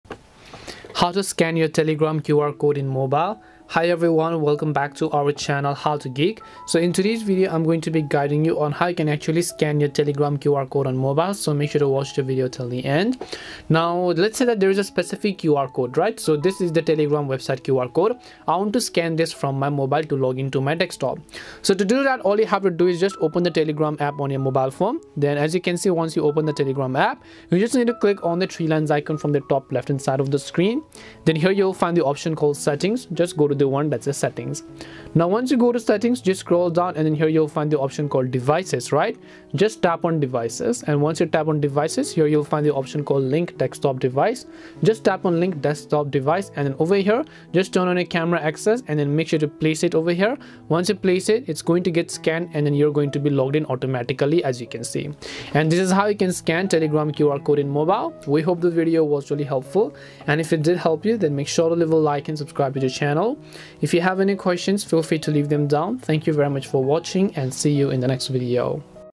How to Scan Telegram QR Code in Mobile !
In this tutorial video, I will quickly guide you on how you can scan the Telegram QR code on mobile. So make sure to watch this video till the end. We try to be responsive to as many questions as possible.

►  Chapters:
0:00 Introduction
0:12 Scan Telegram QR Mobile
0:55 Conclusion